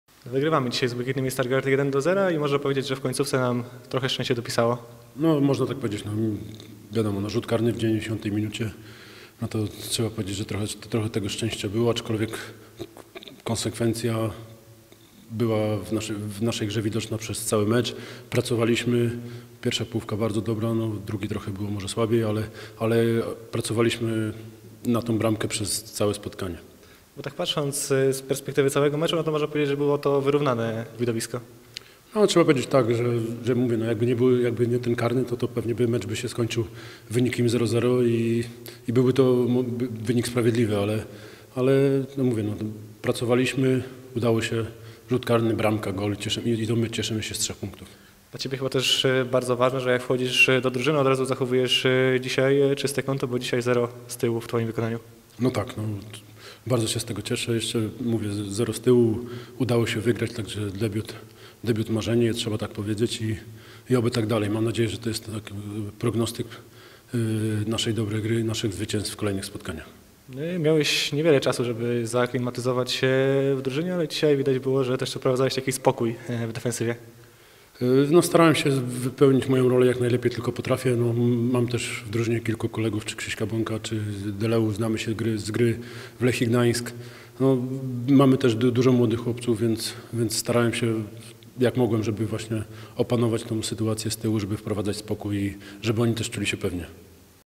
Kapsa: Było trochę szczęścia
-Starałem się, jak mogłem, żeby opanować sytuację z tyłu i wprowadzać spokój, żeby oni też czuli się pewnie - powiedział po meczu z Błękitnymi Paweł Kapsa.